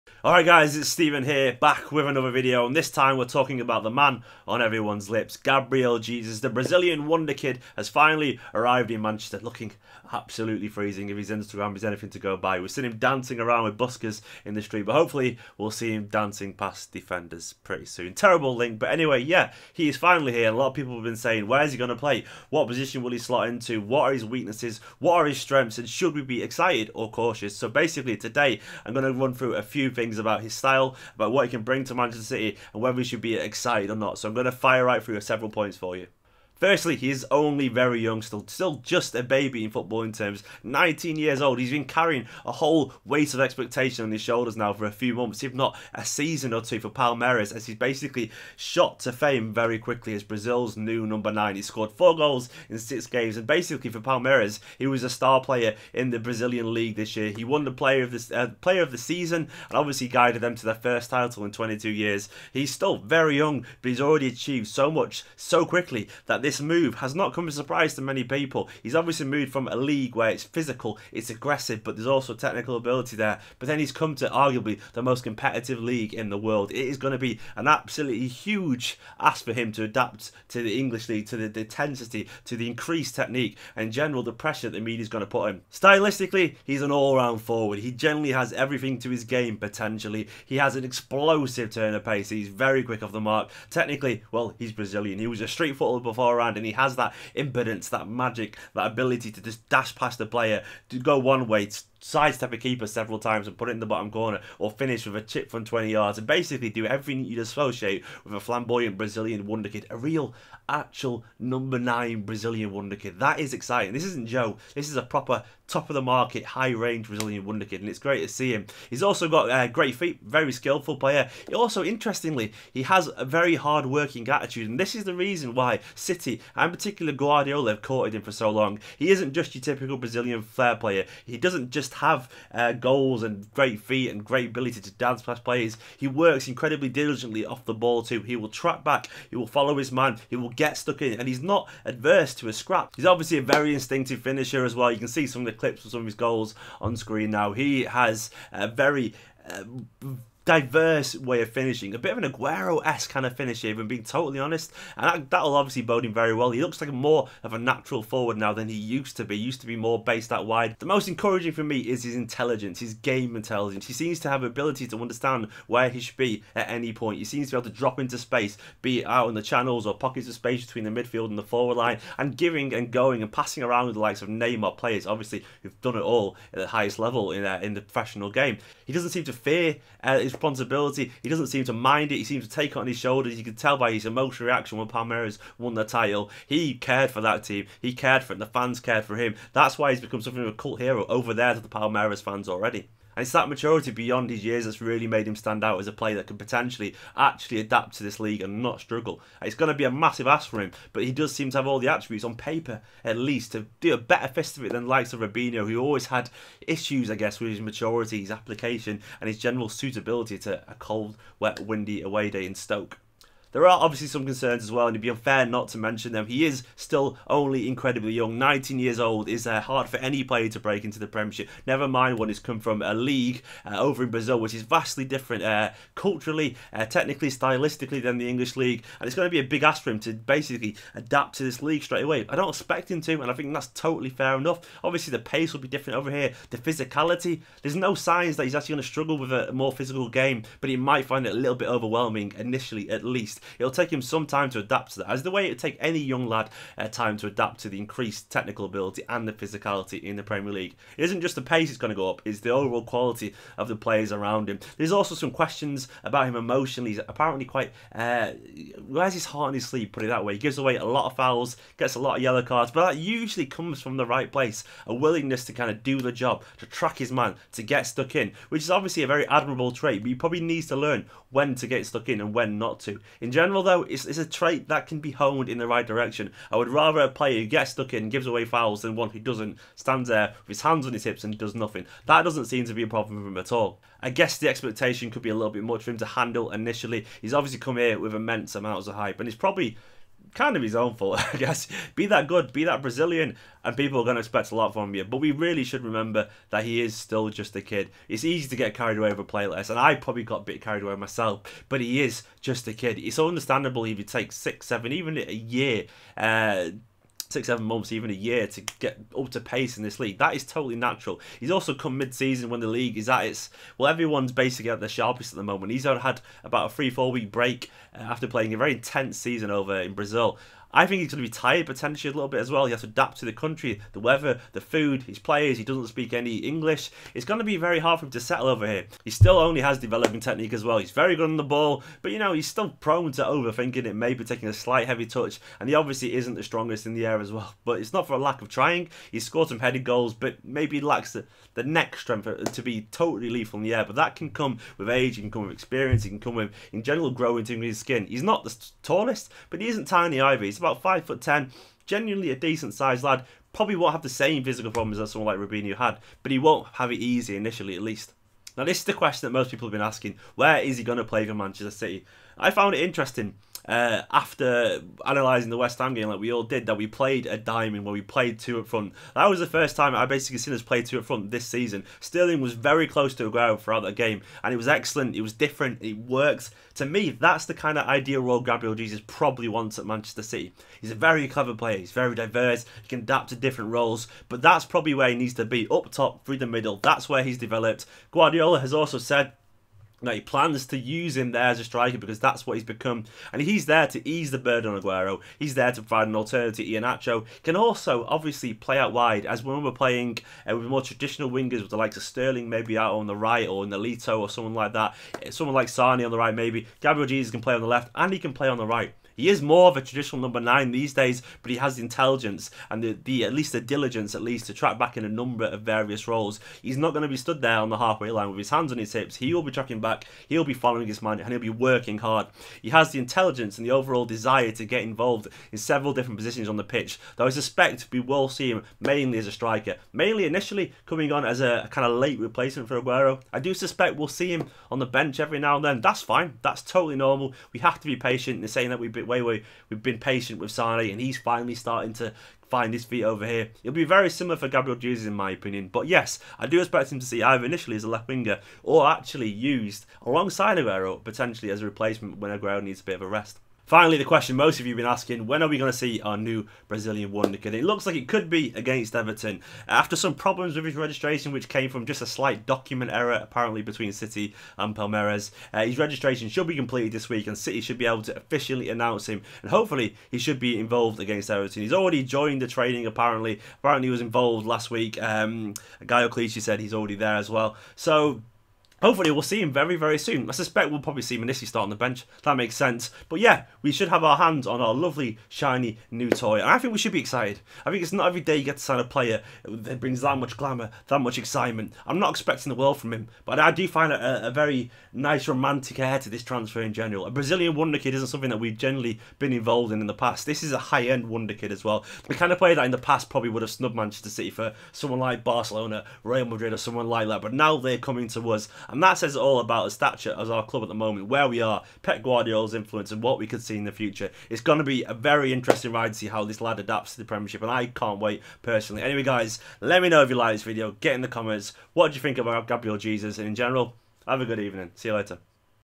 Where Will Gabriel Jesus Play For Man City?
Manchester City's new star has arrived! Gabriel Jesus is here and the question is now how Pep Guardiola will use him at City and whether he will adapt to the Premier League. I discussed his strengths, weaknesses, whether he'll adapt, where he'll play and more. enjoy!